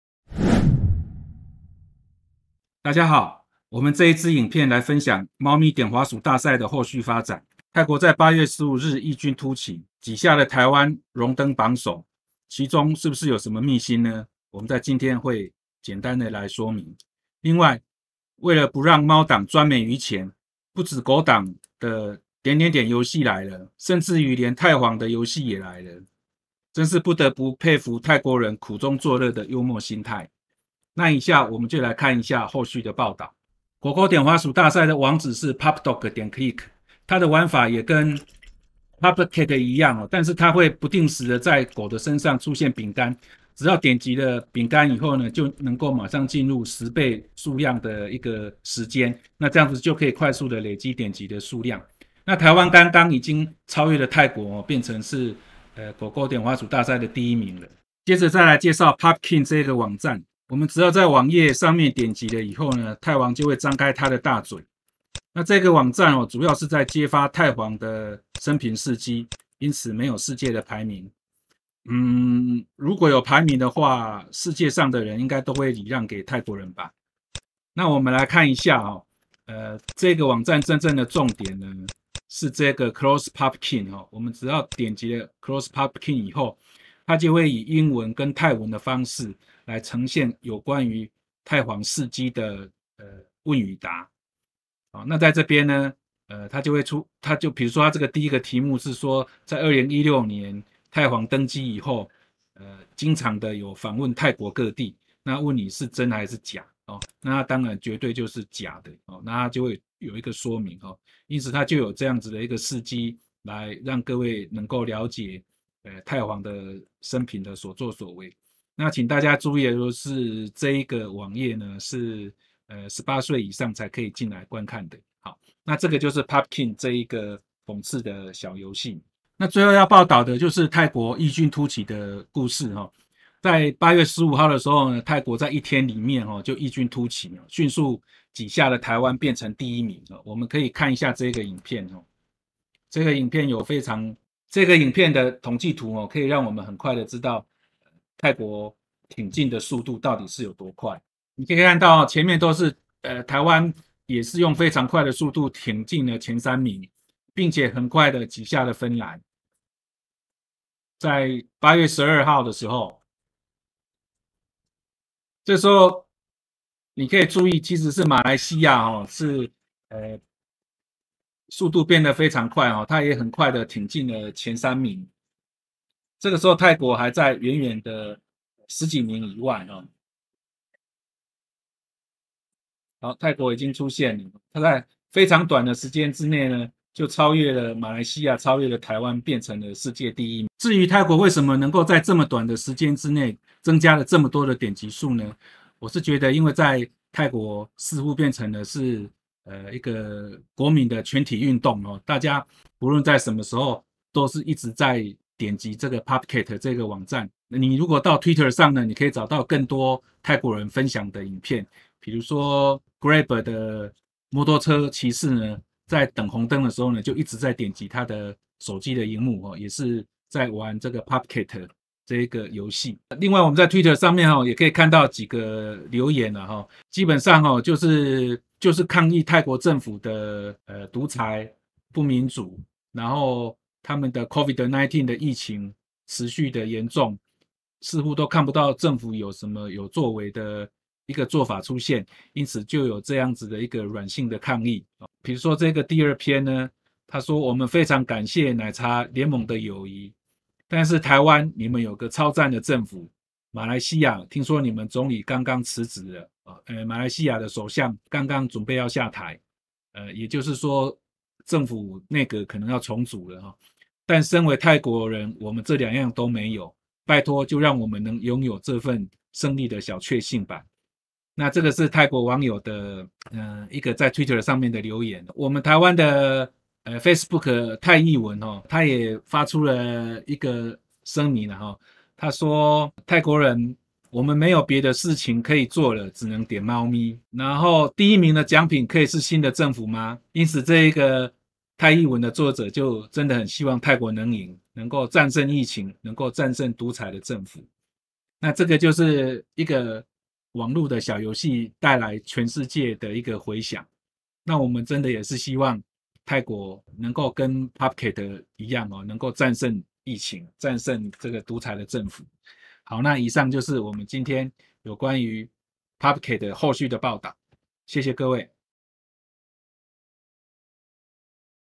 popcat後續報導：狗狗和泰皇也來了！popdog and popking
泰國竄升第一名有何秘辛？
怎麼能讓貓黨專美於前？因此狗黨的點點點大賽也來了。而且不只狗狗來了，「顧人怨」的泰皇也來了。

✅ Facebook 泰譯聞 นักแปล กระแสไทย-ไต้หวัน